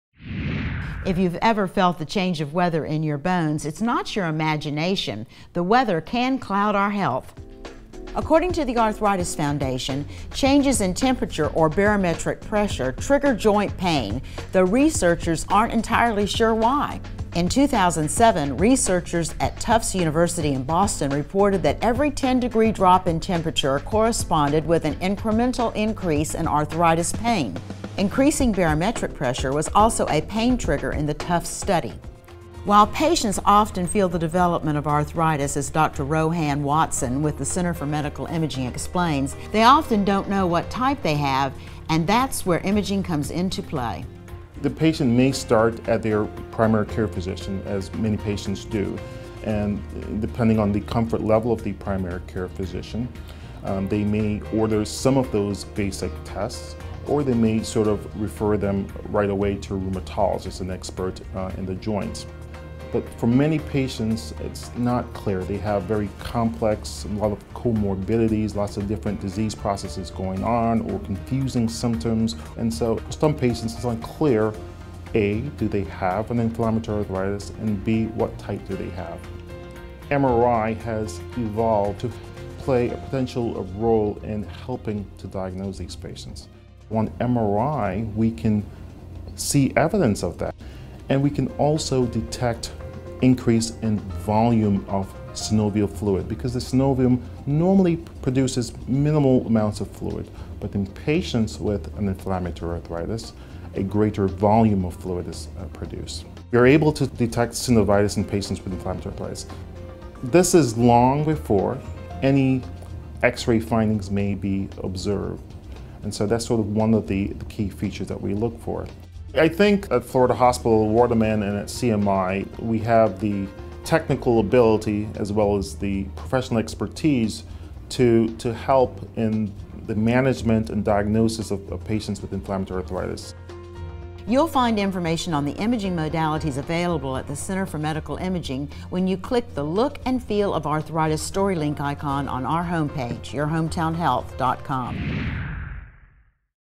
Center for Medical Imaging - Look & Feel of Arthritis
If you’ve ever felt the change in the weather “in your bones”, it’s not your imagination; the weather can cloud your health!

While patients often feel the development of arthritis, as Dr. Rohan Watson with the Center for Medical Imaging explains, they often don’t know what type of arthritis they may have and that’s where imaging comes into play.